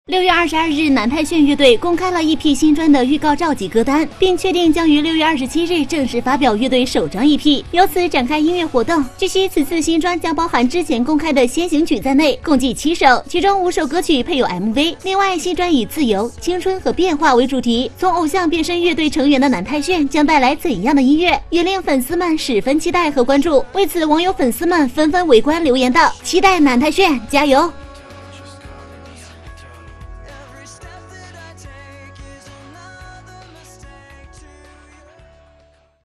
【3年前】南太鉉樂隊27日發表新專 從偶像變身樂隊成員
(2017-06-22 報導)

6月22日南太鉉樂隊，公開了EP新專的預告照及歌單,并確定將于6月27日正式發表樂隊首張EP,由此展開音樂活動.據悉此次新專將包含之前公開的先行曲在內，共計7首,其中5首歌曲配有MV,另外新專以"自由、青春和變化"為主題,從偶像變身樂隊成員的南太鉉將帶來怎樣的音樂,也令粉絲們十分期待和關注。